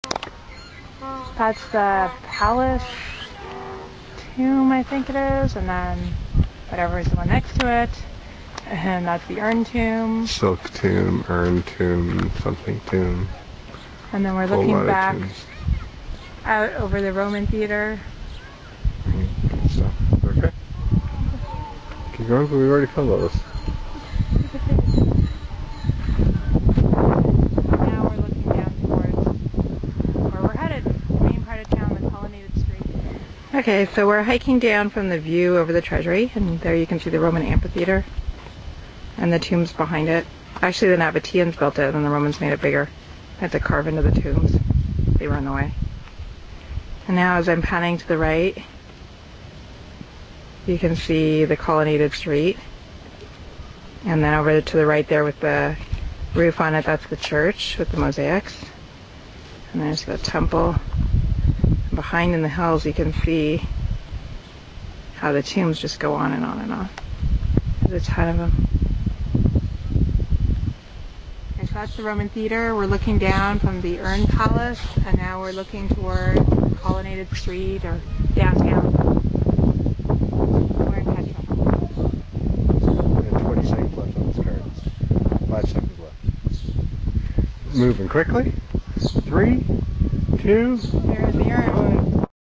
Petra
Vistas panorámicas de Petra. Petra (en árabe, البتراء al-Batrā´) es un importante enclave arqueológico en Jordania, y la capital del antiguo reino nabateo. El nombre de Petra proviene del griego πέτρα que significa piedra, y su nombre es perfectamente idóneo; no se trata de una ciudad construida con piedra sino, literalmente, excavada y esculpida en la piedra.